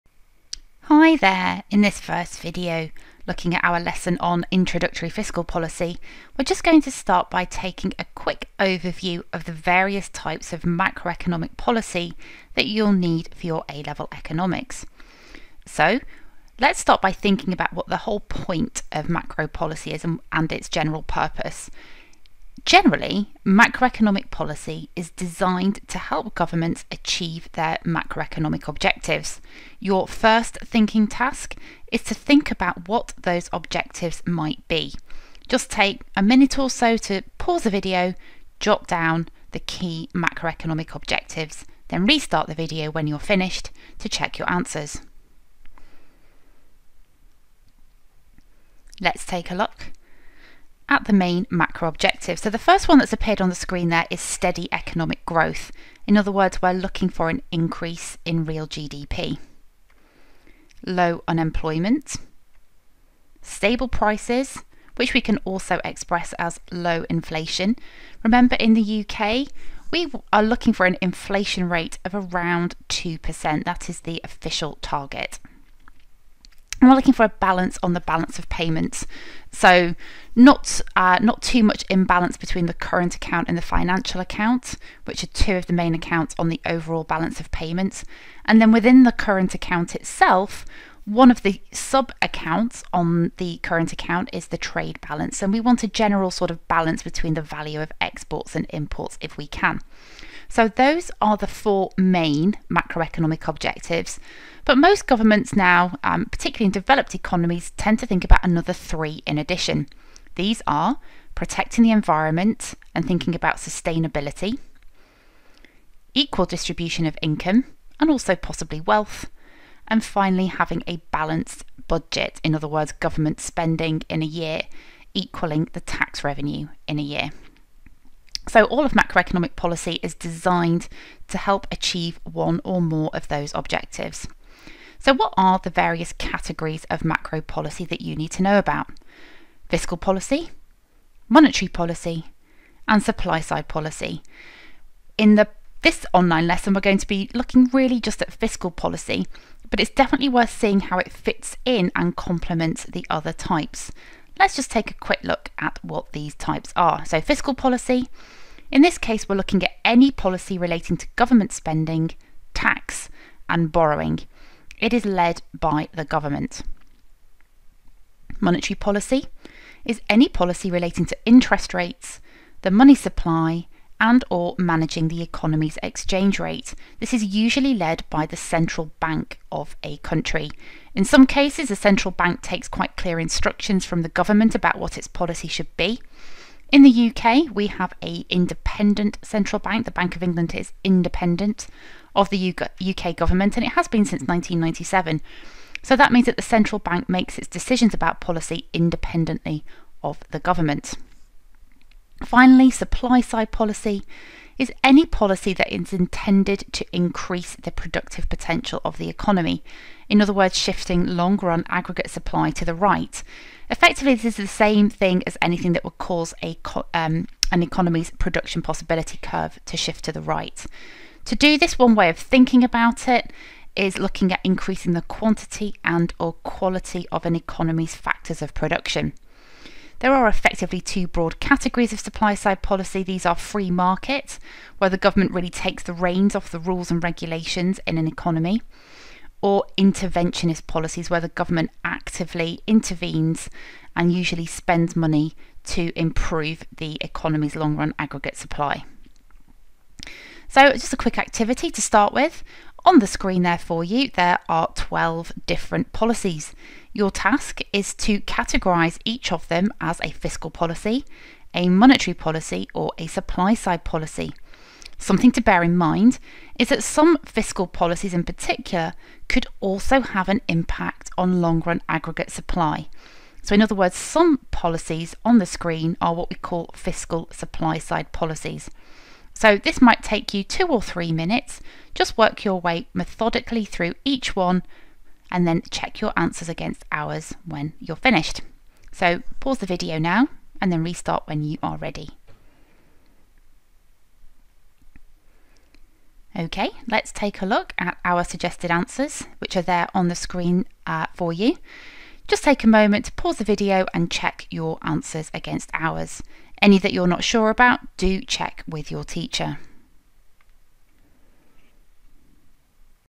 Fiscal Policy - Introduction to Macroeconomic Policies | Economics Revision
In this video we work through the meaning of the 3 main categories of macroeconomic policy: fiscal, monetary, and supply-side, as well as a quick reminder of the main government macroeconomic objectives.